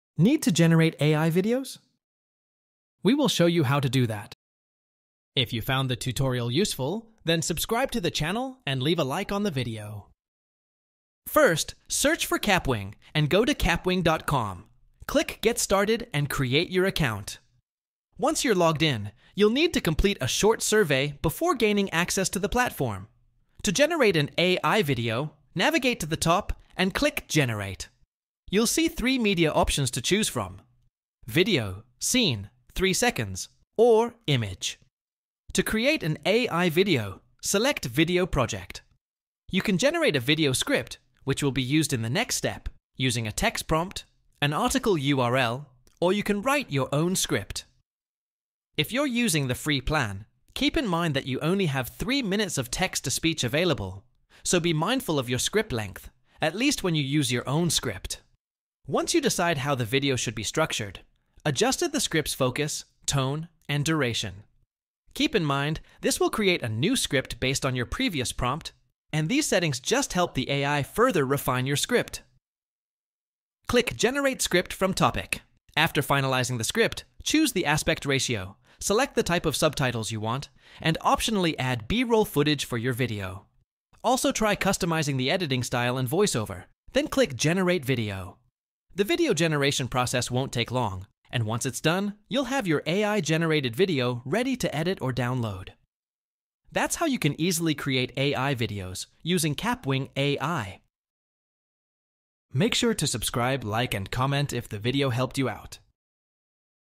How To Create AI Videos Using Kapwing | Kapwing AI Video Generator
Want to learn how to create AI videos using Kapwing? This tutorial will walk you through the entire process of using the Kapwing AI Video Generator, from signing up to generating professional-looking AI-powered videos. Whether you're using a custom script, an article URL, or AI-generated content, Kapwing makes it easy to create engaging videos with text-to-speech, subtitles, and B-roll. We’ll also cover how free users have a 3-minute text-to-speech limit and how to optimize your video for the best results. Watch this full guide to start making AI-generated videos with Kapwing AI today!

📣 Share your thoughts or suggest tutorial topics in the comments below, and let's discuss the amazing world of tutorials!

Credit to ElevenLabs for the voice in the video.
If you want to use the same voice as in the video, check ElevenLabs out: